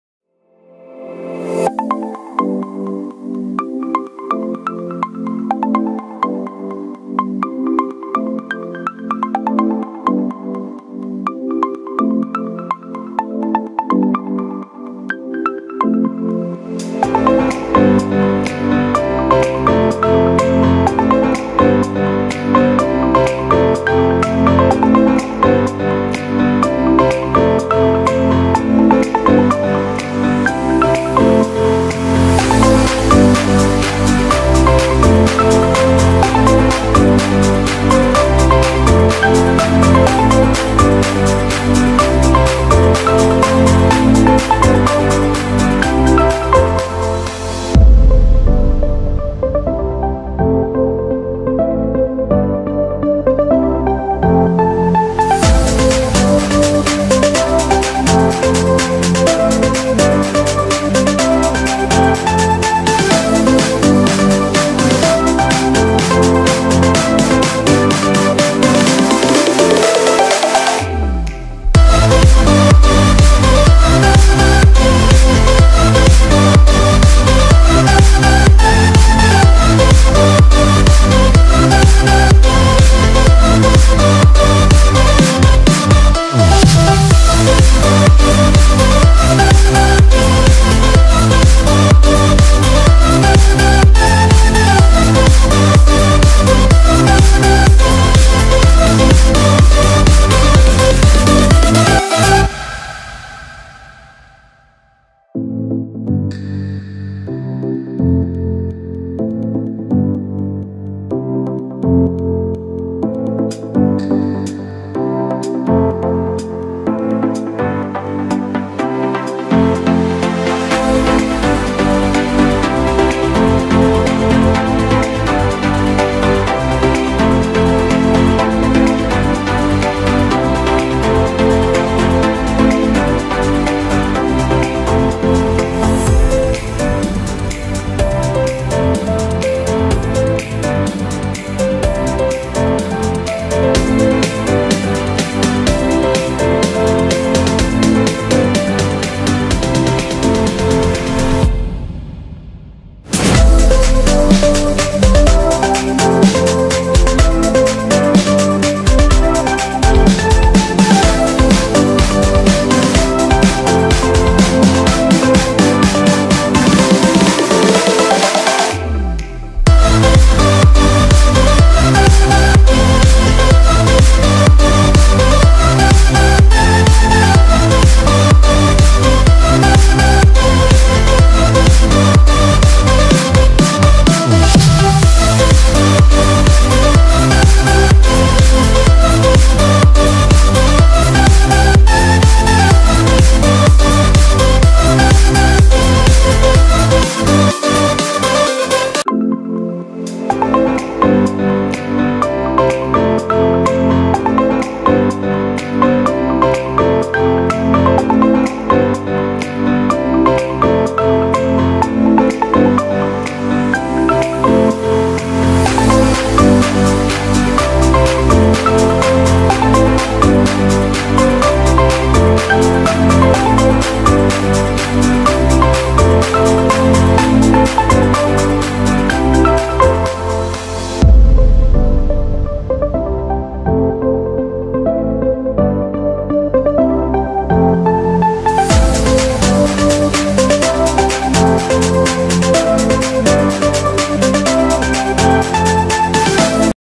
C LANGUAGE PART-3||How to install turbo c
installation of turbo c++ for writing c language code